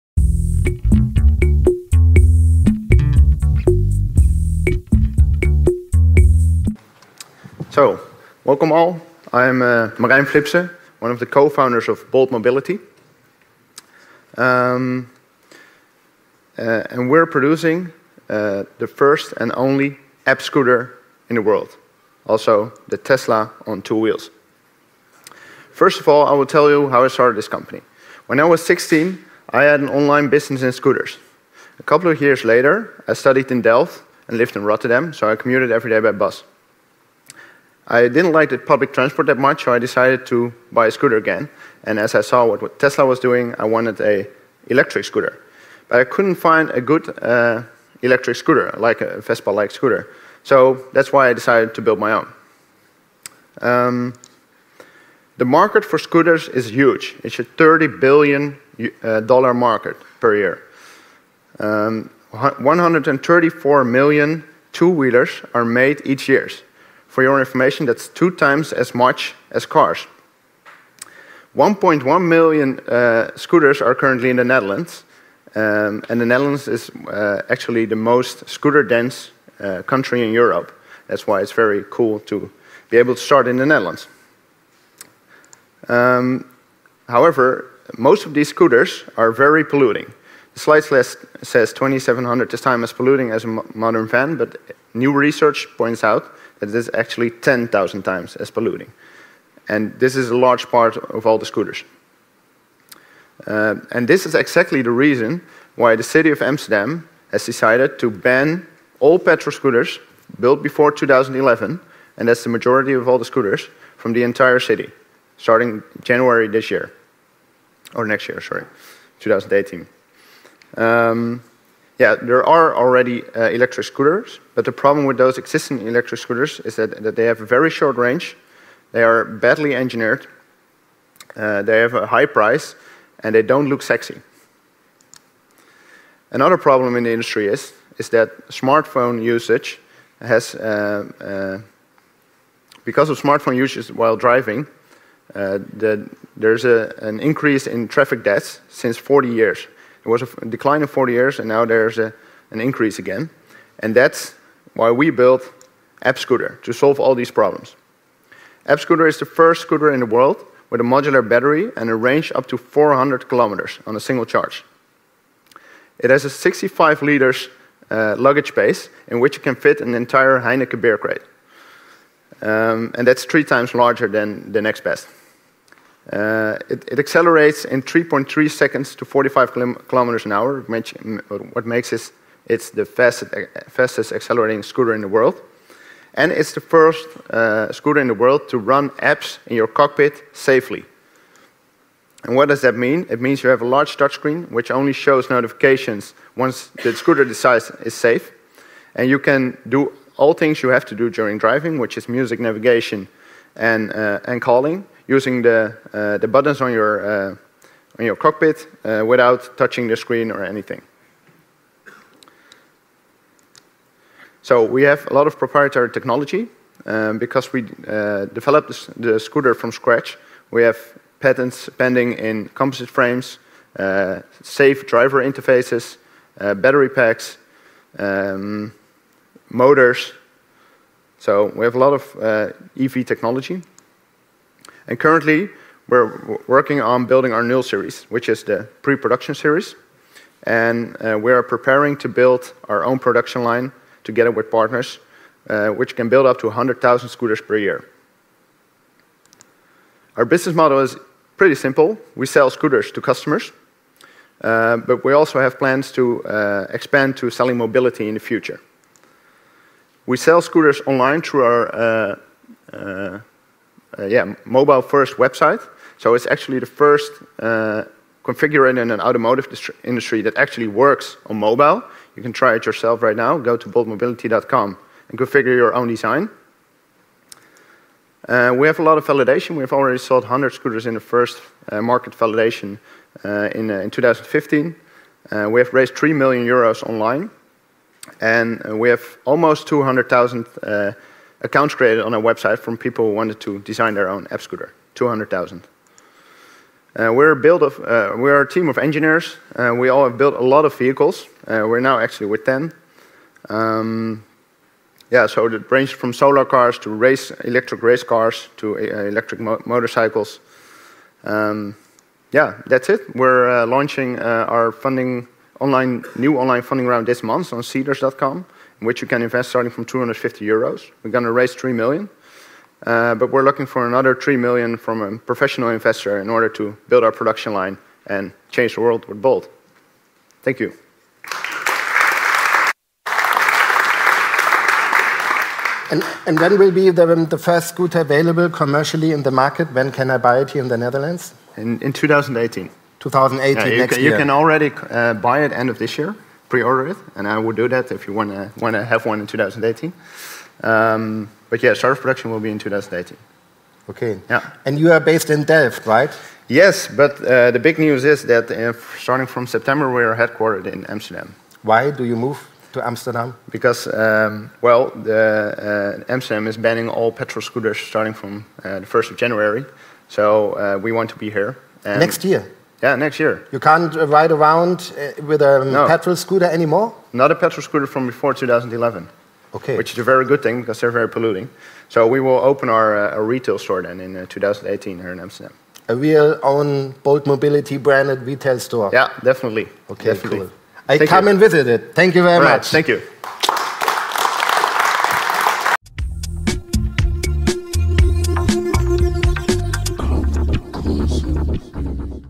ECO17 Amsterdam: Marijn Flipse Bolt Mobility
Lightweight electric scooter: Marijn Flipse pitches Bolt Mobility at Ecosummit Amsterdam 2017.